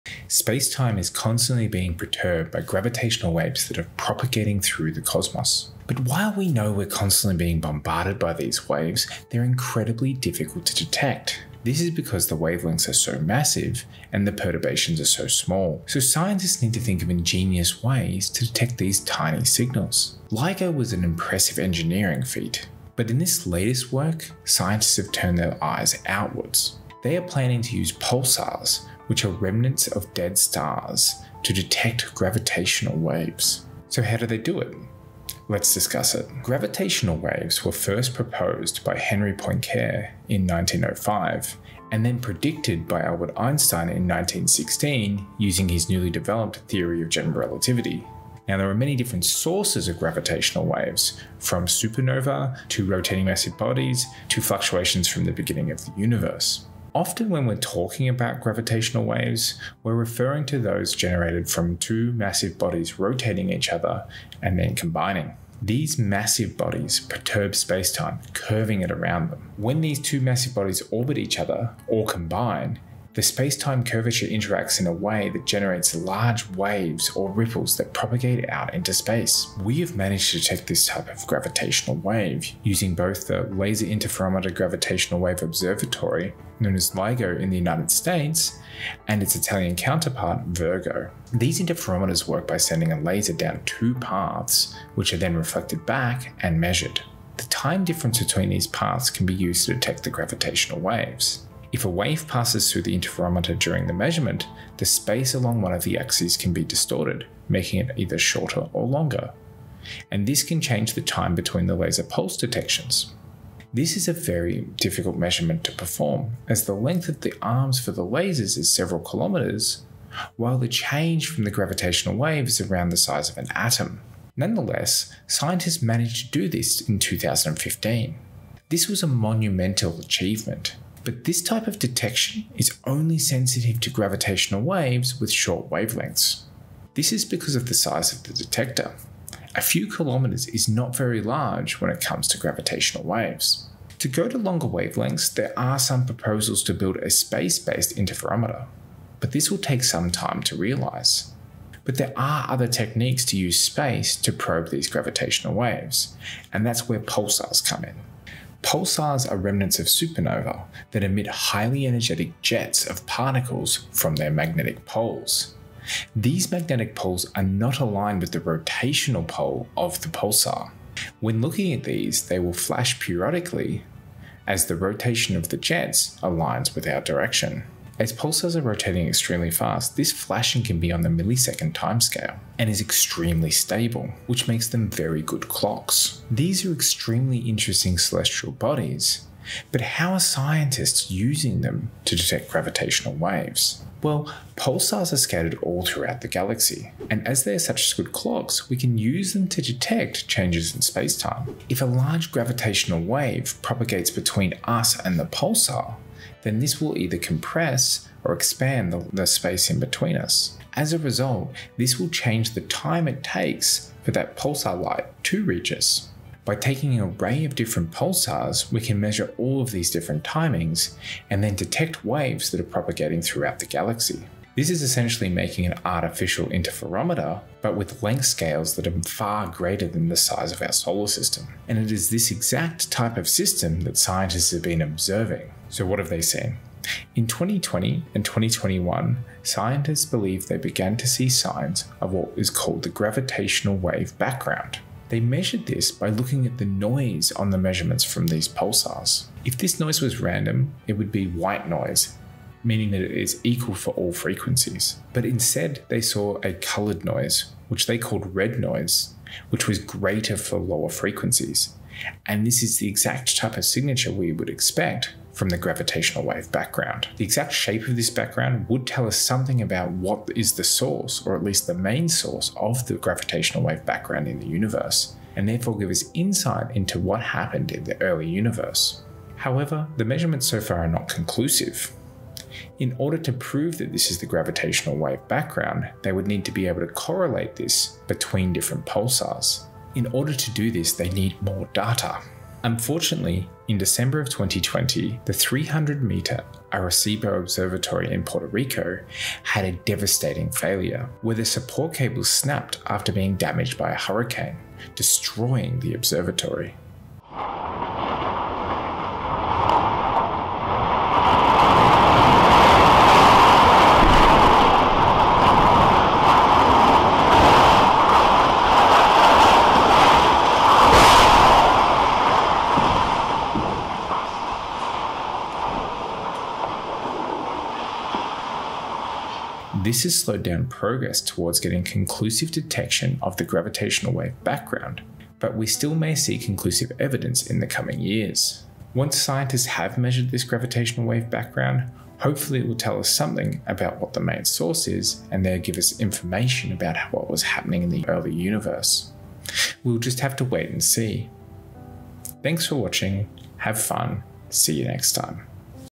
Are Pulsars The Next Generation of Gravitation Waves Detectors
Gravitational waves are an amazing result of General Relativity, but we still know very little about them. This is because of how hard it is to measure them. LIGO and VIRGO have demonstrated that gravitational waves do exist, and we can measure them. But they are limited to measurements of short wavelength gravitational waves. To extend this to larger wavelengths the size of the experiment must become much larger. Eventually, we will have spaced-based interferometers, but we are not there yet. An alternative method, that looks at longer wavelengths again, is to use pulsars. In this video, I describe some of the background behind how these measurements work.

Sources
Paper

Other media
There is some great article written on this topic in Nature:

Also check out this video by PBS Space Time that also covered this topic in more detail